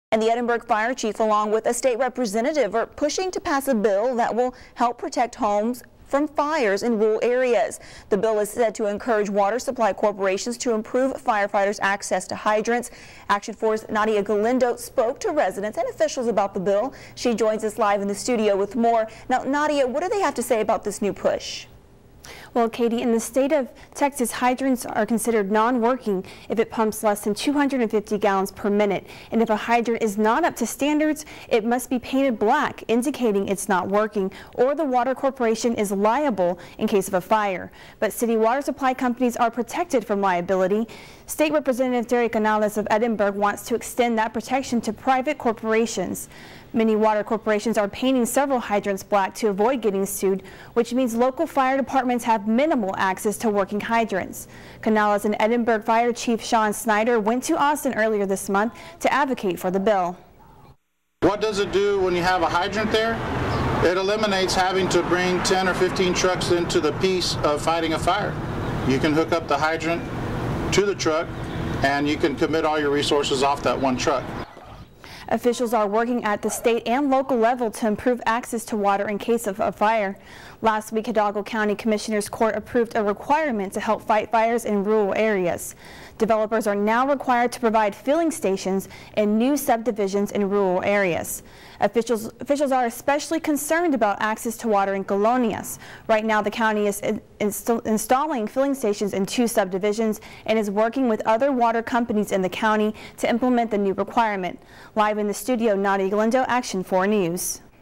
In Case of Fire: Officials hoping to improve water access in rural areas
The Edinburg fire chief along with a state representative are pushing to pass a bill that will help protect homes from fires in rural areas. The bill is said to encourage water supply corporations to improve firefighters access to hydrants.